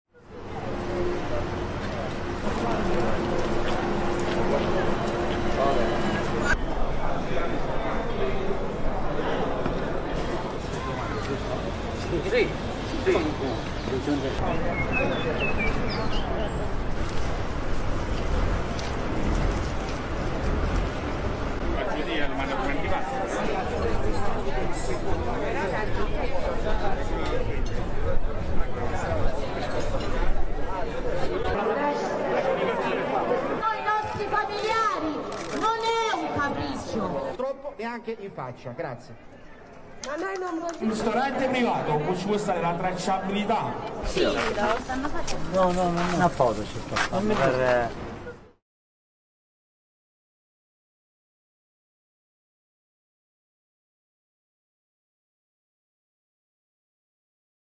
Italy reopens restaurants and cultural attractions as COVID restrictions eased - News 360 Tv
Locals were seen gathering at restaurants and cultural sites in Rome on Monday as restrictions put in place due to the coronavirus pandemic were eased.

Restaurants and bars were limited to outdoor service only, while archaeological sites and museums were allowed to receive visitors once more, with theatres and cinemas also reopening their doors albeit with capacity limits and social distancing requirements.

One restaurant worker welcomed the move, saying "I hope this is the last lockdown and that they give us the opportunity to always be open. It is the most important thing for the sector." Another man shared his joy that outdoor dining was back, explaining that he was 'very happy' about the opportunity.